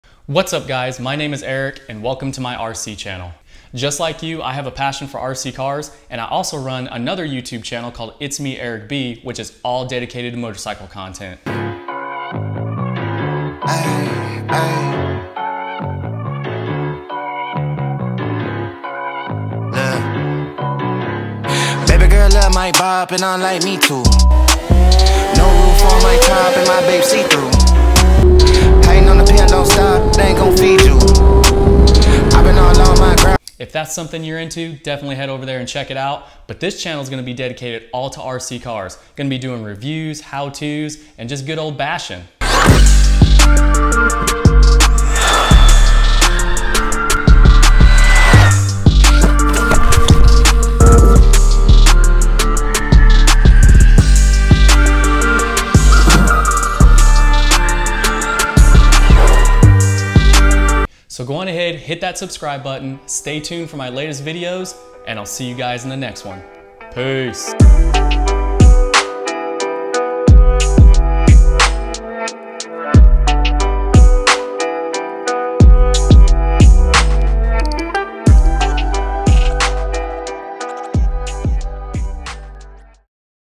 WELCOME TO MY RC CHANNEL! | Intro to my ARRMA TYPHON 3S
Hello everyone!  This is my new YouTube channel dedicated to everything RC!  I run another YouTube channel called @ItsMeEricB and it's focused on motorcycle content.  I am not new to RC, but I did take a break for about 6 years.  Now I'm back!  I love RC cars, helicopters and planes!  Lastly, I am also a filmmaker and I love to tell stories though my videos.  🙂